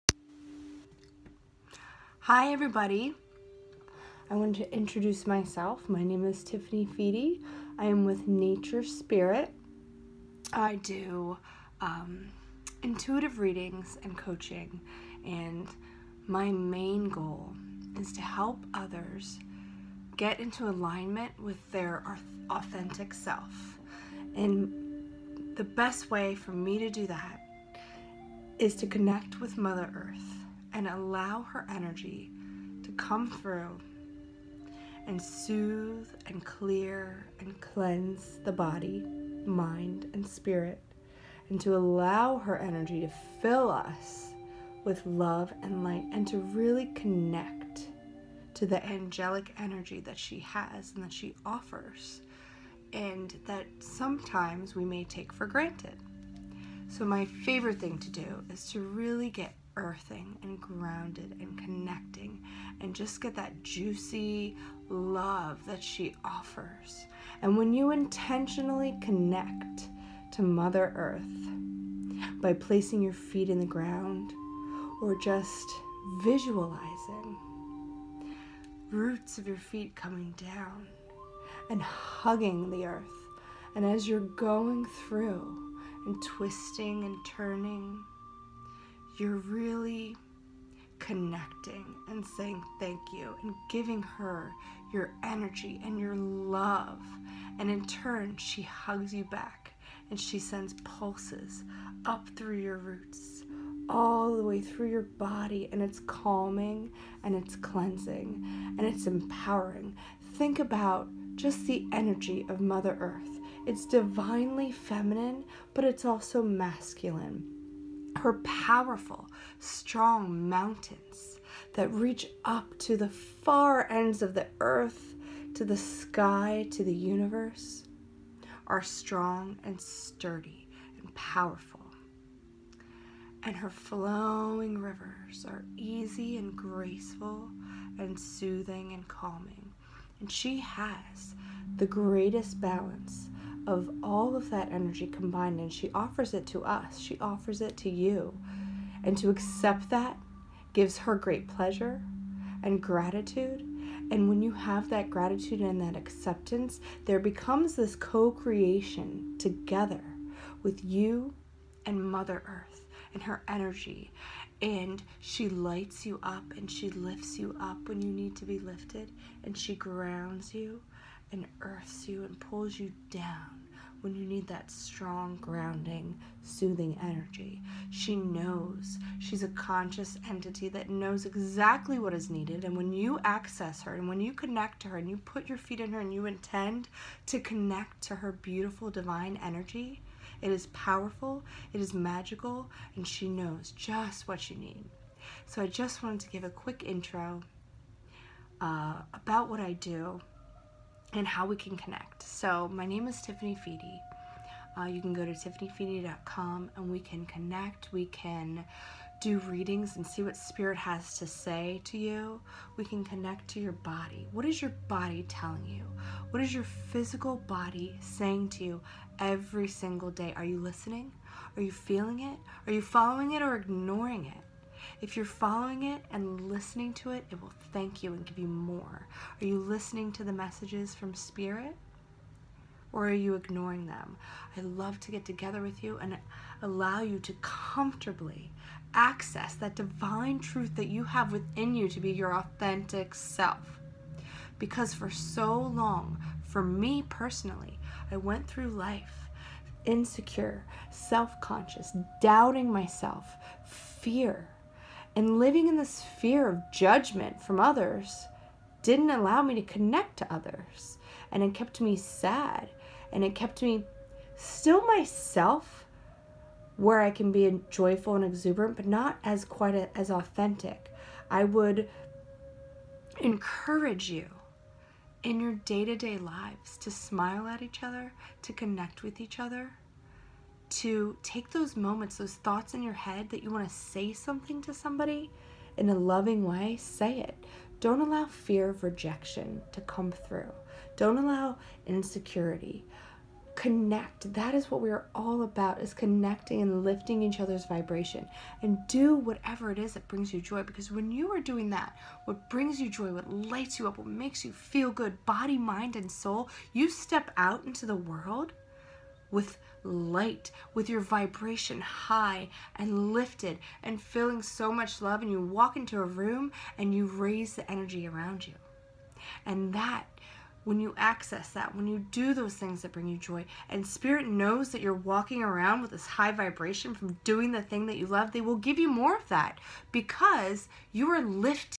Lift up that Vibration Y’all !
Tiffany Fede, PsychicIntuitive, energy healer, GAIA energy.

Earth energy healings
Intuitive readings

In this video I’m introducing you to what I’ve been co-creating with spirit. I bring forth Gaia energy and messages of how you can access her love.